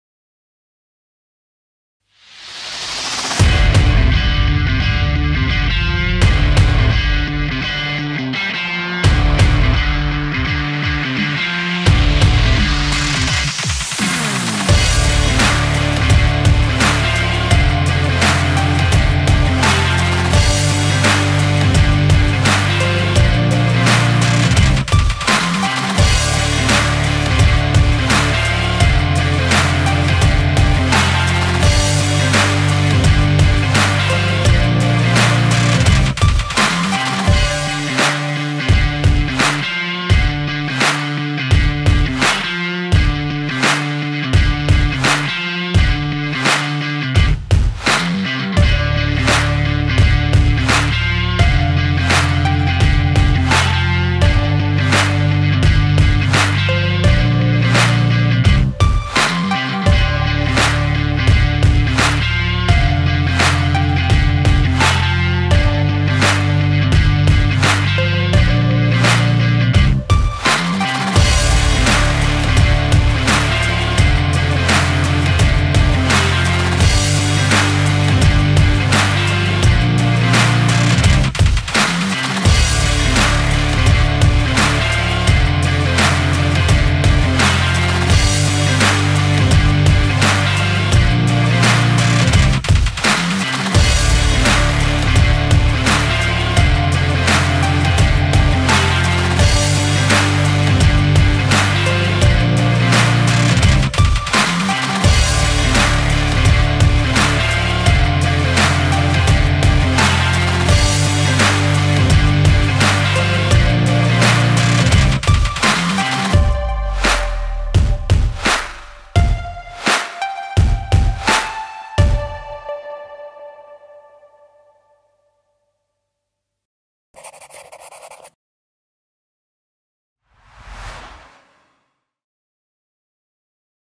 Chantier Lève Toit, Fouday, Octobre 2020
Présentation d'un chantier hors normes, par la technicité mise en oeuvre. 3 Grues pour la mise en place de vitrages.